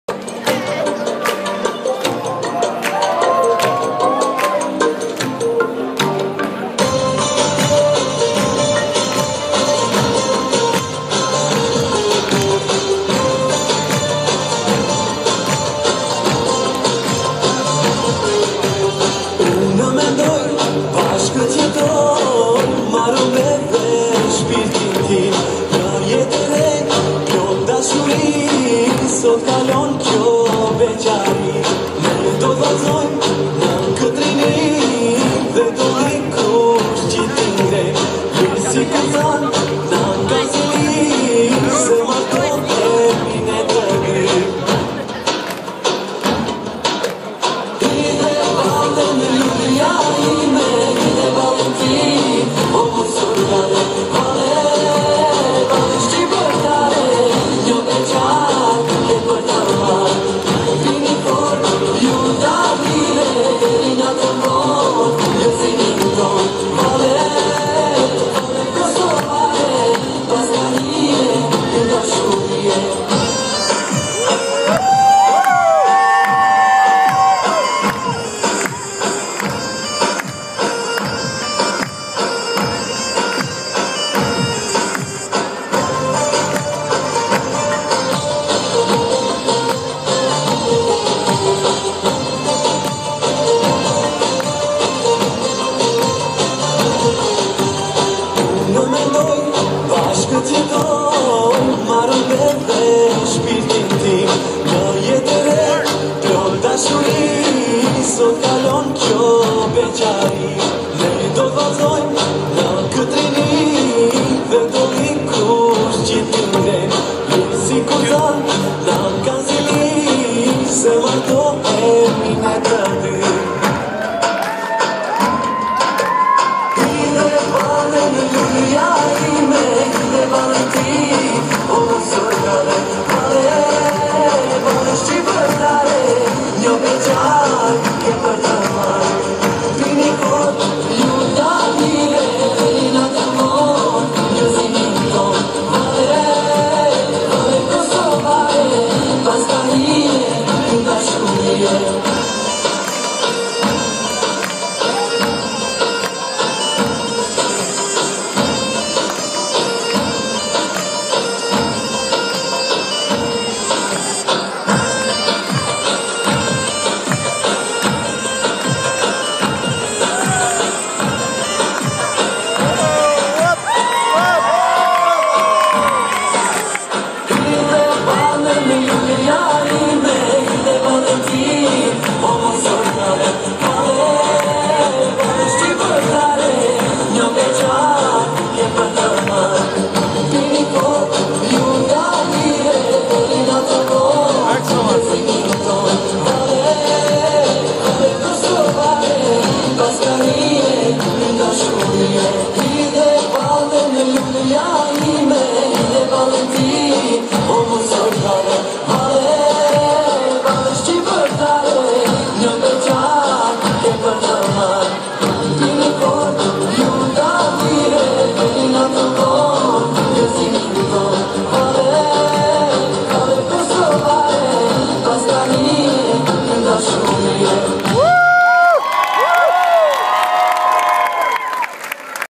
Valle Kosovare kercejnë fëmijet
Fëmijet kercejnë per bukuri per festen e 101 vjetorit të Kryengritjes së Malësisë në Sallën e Kishes së Shen Palit, Rochester Hills Michigan!